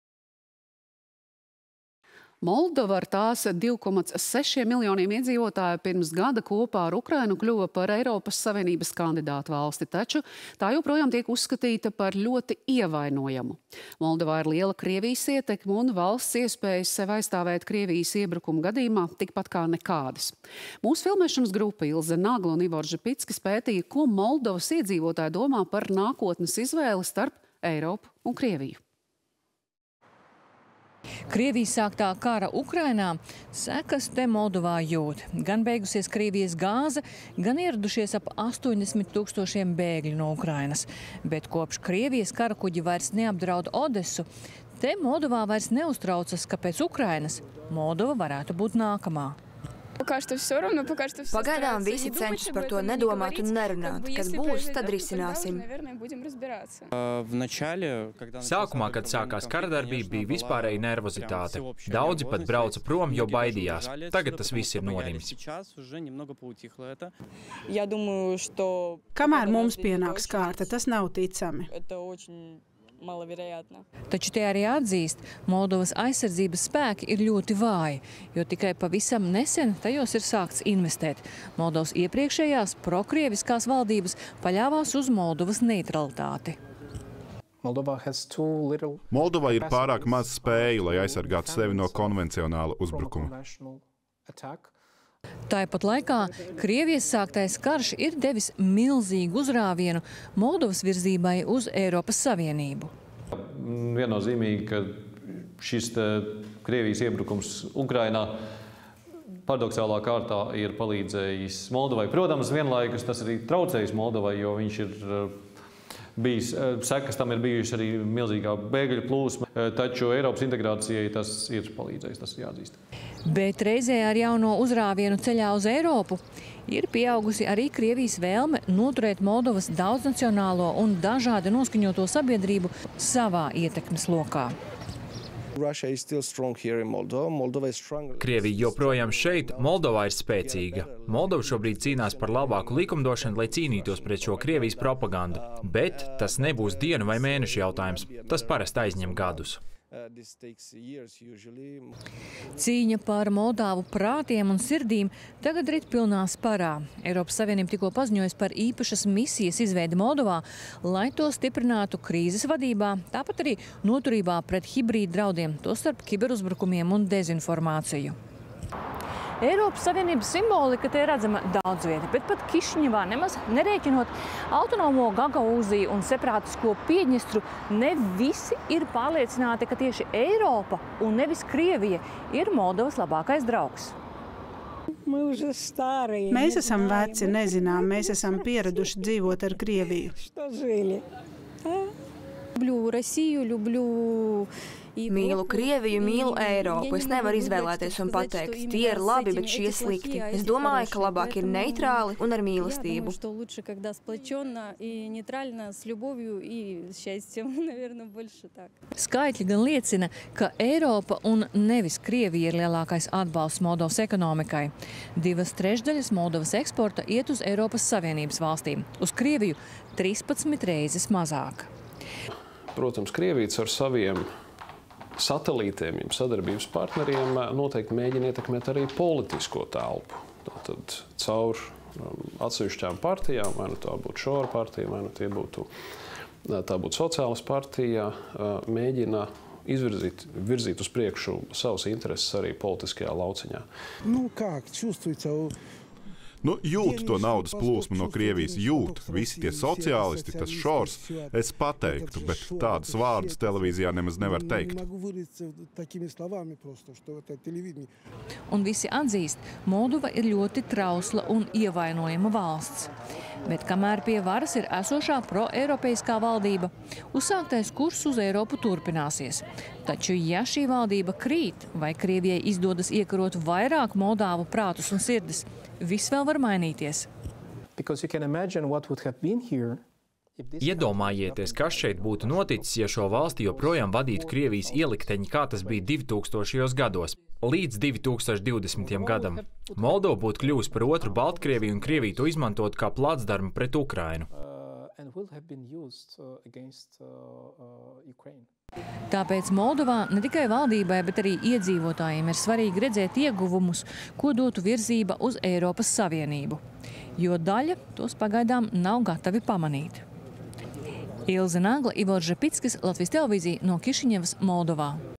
Moldovā – cīņa par drošību, prātiem un sirdīm
Moldova ar tās 2,6 miljoniem iedzīvotāju pirms gada kopā ar Ukrainu kļuva par ES kandidātvalsti. Taču tā joprojām tiek uzskatīta par ļoti ievainojumu. Moldovā ir liela Krievijas ietekme un valsts iespējas sevi aizstāvēt Krievijas iebrukuma gadījumā – tikpat kā nekādas. Mūsu filmēšanas grupa – Ilze Nagla un Ivo Ržepickis – pētīja, ko Moldovas iedzīvotāji domā par nākotnes izvēli starp Eiropu un Krieviju.